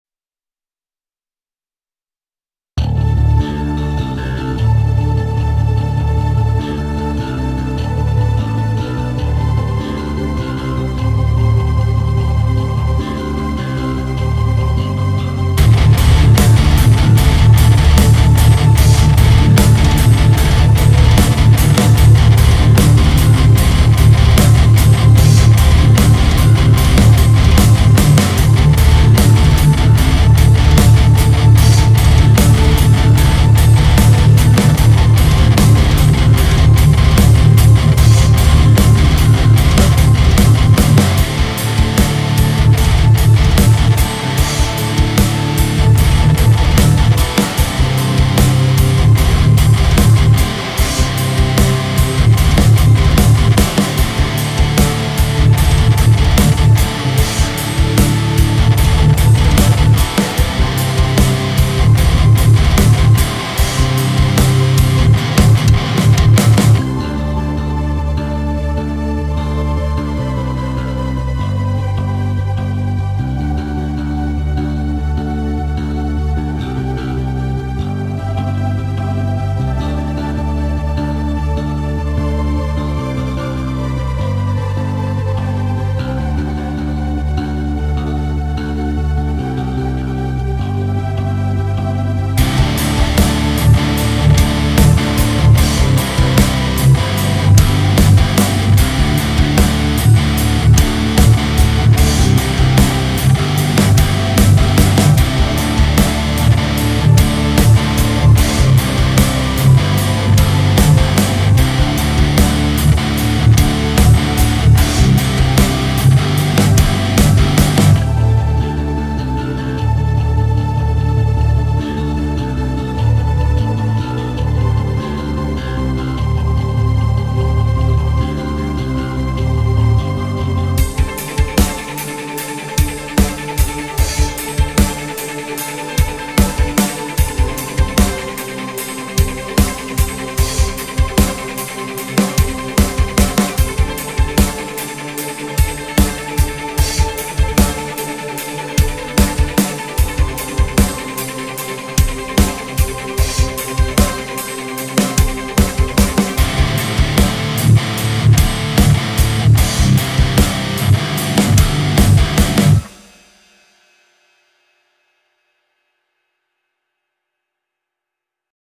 My Shreddage Song 7 - Alternative Metal / Electronic (FL STUDIO MY CREATIONS / SHREDDAGE 2)
Software / Plugins / Instruments :
FL STUDIO 20
- Shreddage IIX
- Shreddage Bass 2
- Shreddage 2 IBZ
- Shreddage Drums
- Guitar Rig 5
- reFX Nexus 2